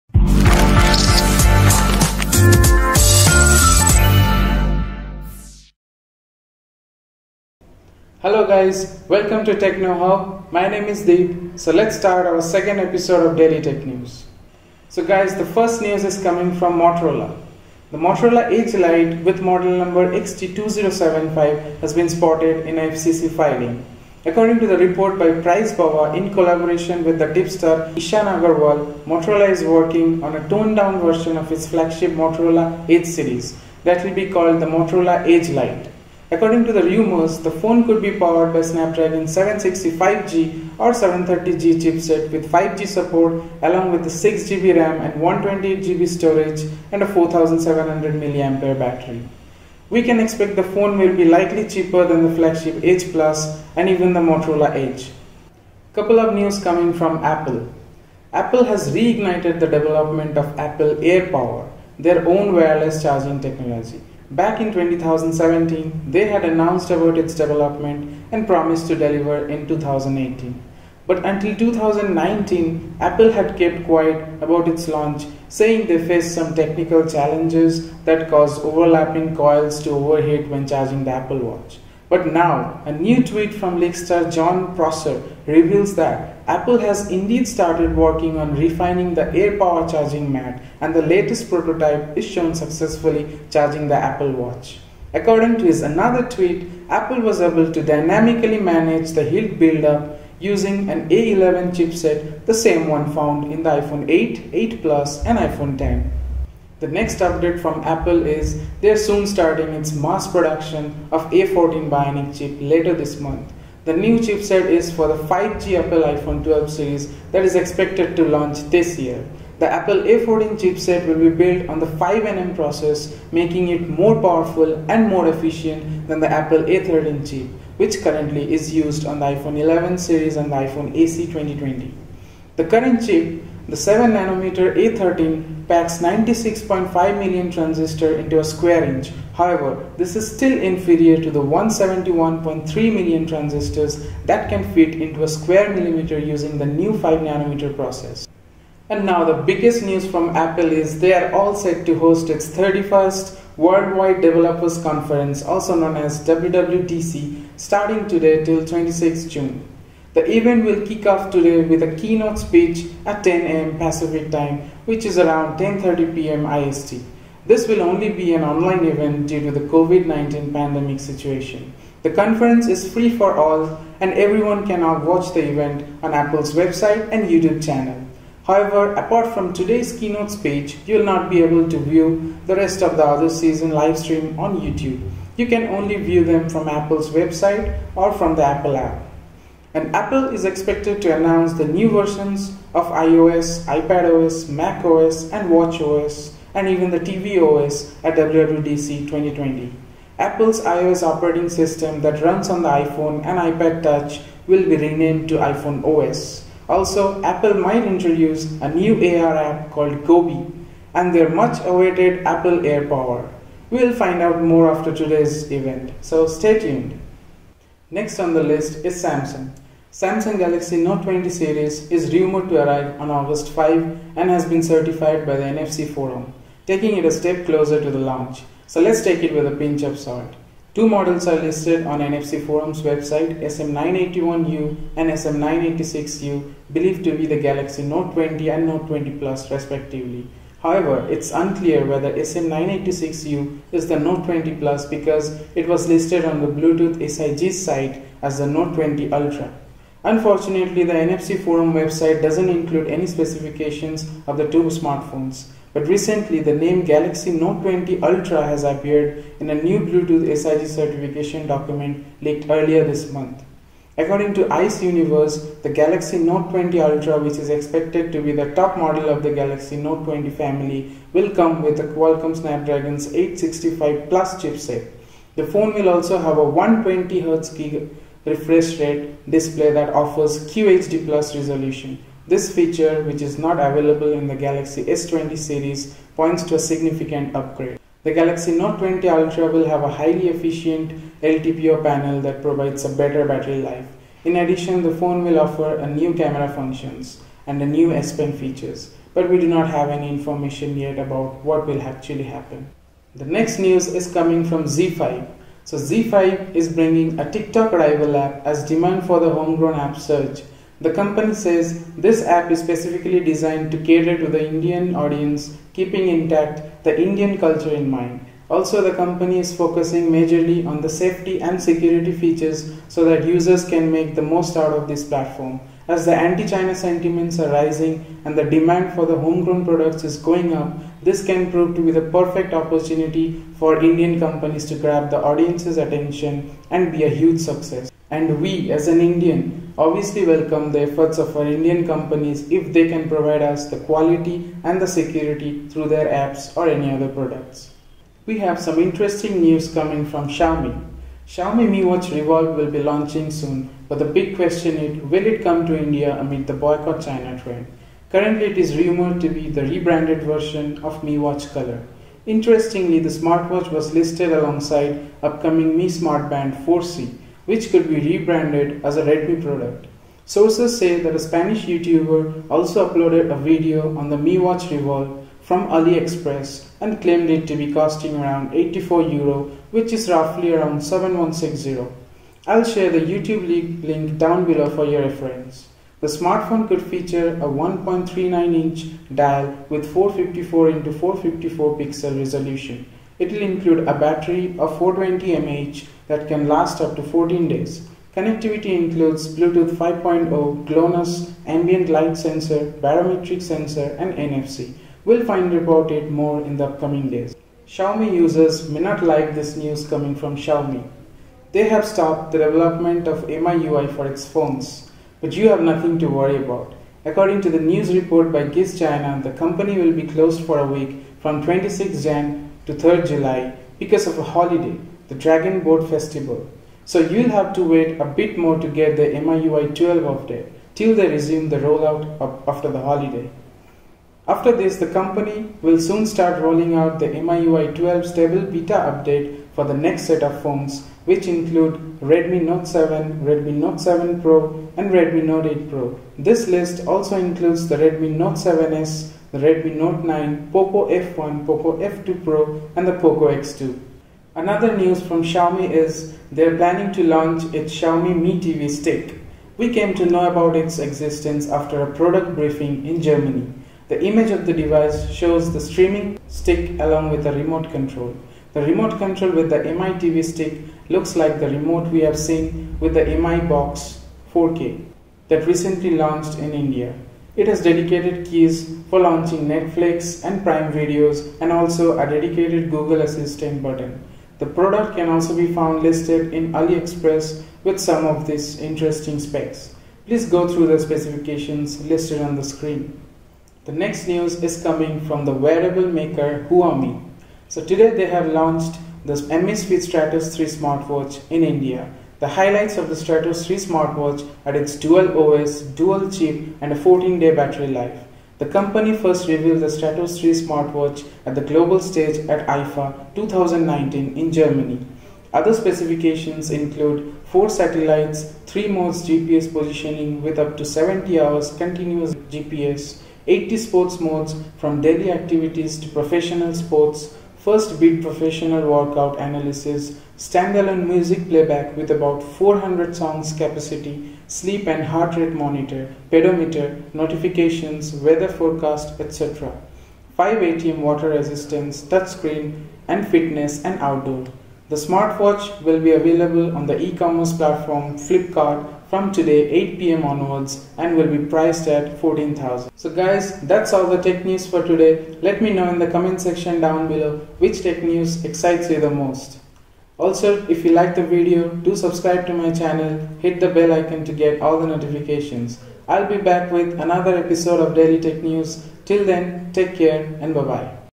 Technews | Apple WWDC 2020+Airpower+Gobi 🔥 Motorola Edge Lite 😍 Mi TV Stick 😱 Tiktok Rival!😱 Note 20
Welcome to the 2nd Episode of Daily Tech News!

In this video, I have discussed about the newly launched Motorola Edge lite, Apple WWDC 2020 event, Apple airpower, apple app Gobi, Apple a14 Bionic chip.
I have also updates from samsung, that they are going to launch Galaxy Note 20 series.
Zee 5 is bringing their Rival TikTok app.
Xiomi is launching its Mi watch Revolve and Xiomi Mi TV stick.
Huami has launched its smartwatch stratos 3 in flipkart.

Xiaomi Mi Watch Revolve link by spanish youtuber -